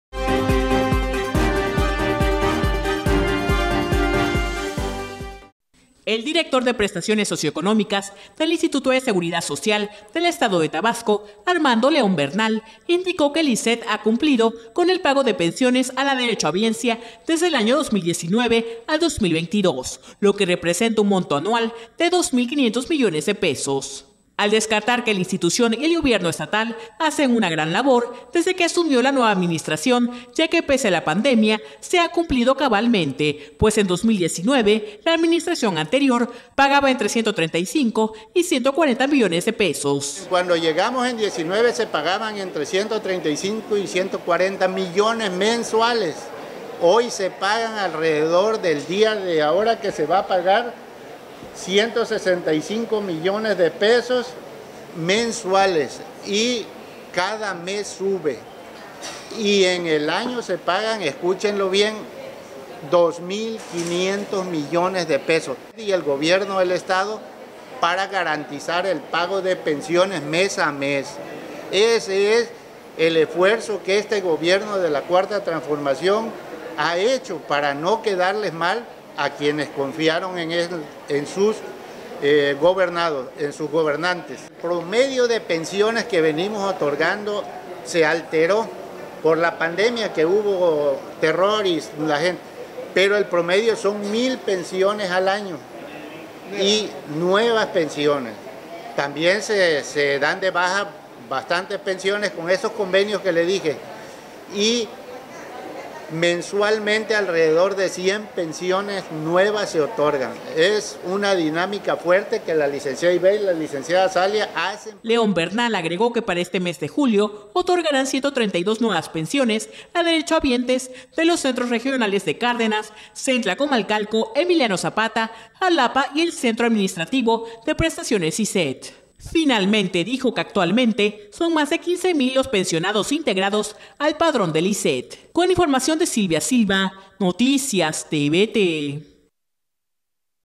Cumple ISSET con el pago de pensiones a la derechohabiencia.
NoticiasTVT🟣 Para garantizar el pago de pensiones cada mes a 15 mil derechohabientes, el ISSET eroga anualmente de dos mil 500 millones de pesos, precisa el director de Prestaciones Socioeconómicas, Armando León Bernal.